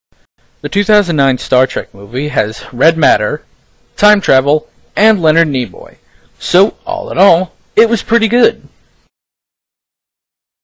10 Second Review - "Star Trek (2009)"
Great Movie!

Credits:
Writer: SirRoland64
Director: SirRoland64
Performer: SirRoland64
Editor: SirRoland64
Star Trek is owned by Paramount Pictures.